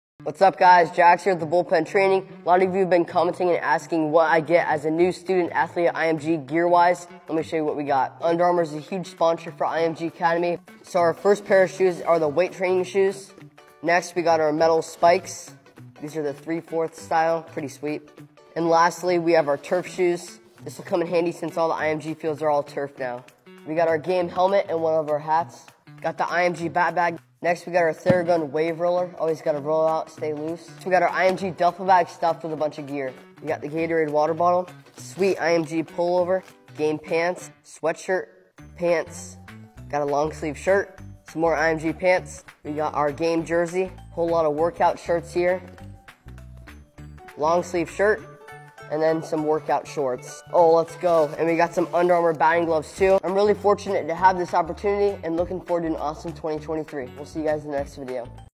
UnBoxing IMG Academy Swag Bag Gear for a NEW Baseball Student Athlete
Get FREE 2 Day Expedited Shipping with Code " JAXFREE " on the CatX BBCOR Alloy used in this Video